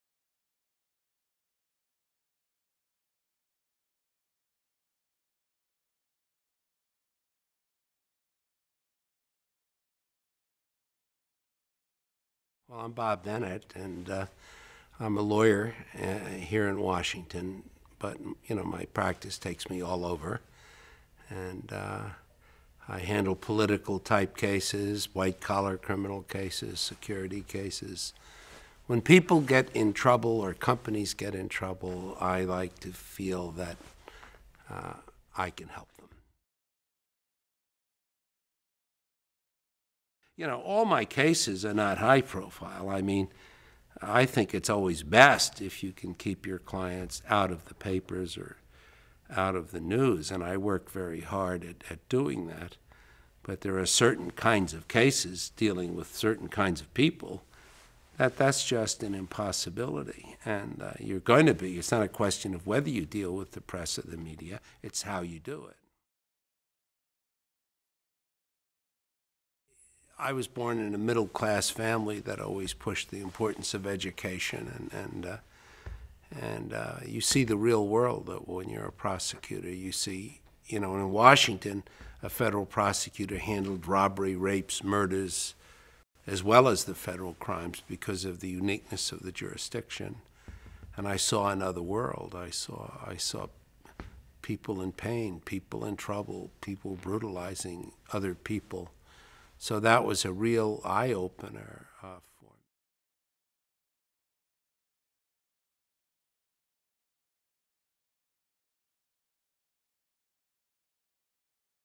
Bob Bennett
"I often think that growing up in the streets of Brooklyn has been more helpful to me in the pugnacious world that we live in than my degrees from Harvard and Georgetown."

Robert S. Bennett is a partner in the Washington, D.C., firm of Hogan Lovells. His extensive experience includes representing two former secretaries of defense, Clark Clifford and Caspar Weinberger, serving as President Bill Clinton's personal attorney in the Paula Jones case, and representing Judith Miller in the CIA leak investigation.